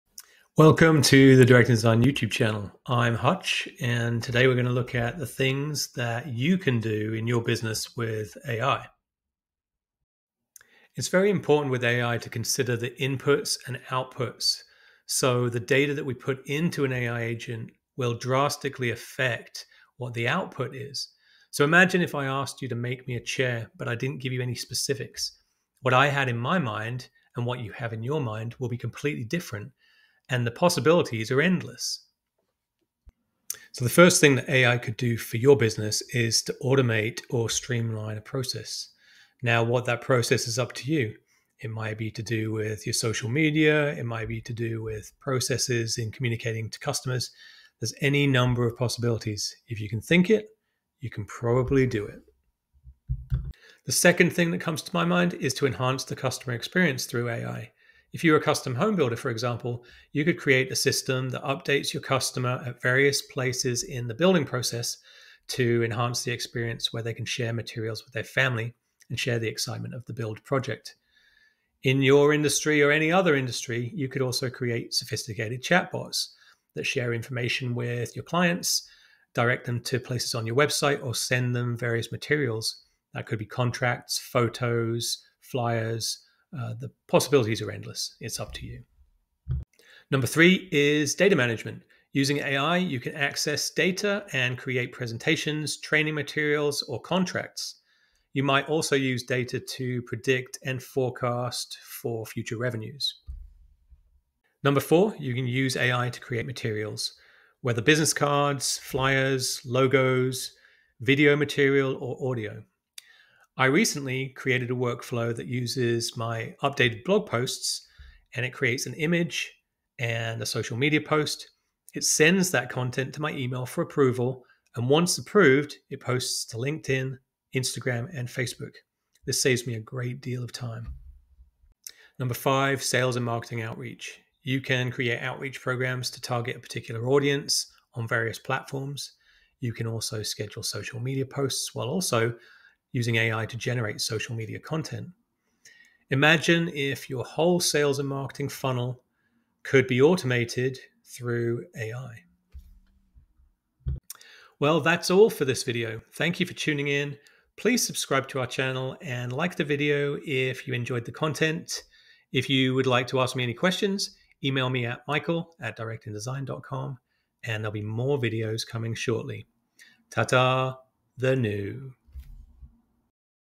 5 Things You Can Do With AI at Work or in Your Business.
A quick dive into AI and what you can do at work or in business with AI.
From content creation to automation. In this video, we don't go into depth regarding the tools you can use but, in simple terms, express ideas about what YOU can do with AI.

00:33 Automation
00:52 Customer Experience
01:29 Data Management
01:44 Create Things
02:12 Sales and Marketing Solutions